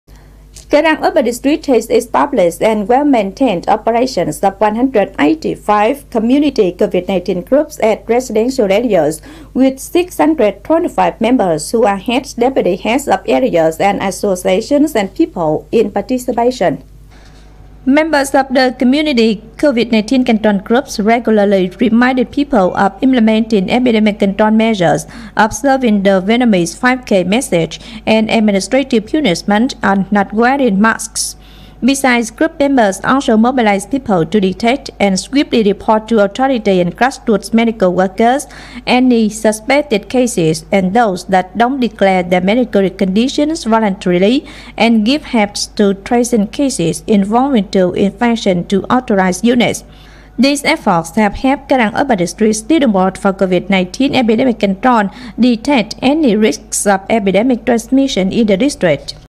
Cái Răng maintained performance of 185 community COVID-19 control groups
Cái Răng urban district has established and well maintained operation of 185 community COVID-19 groups at residential areas with 625 members who are heads, deputy heads of areas and associations, people in participation.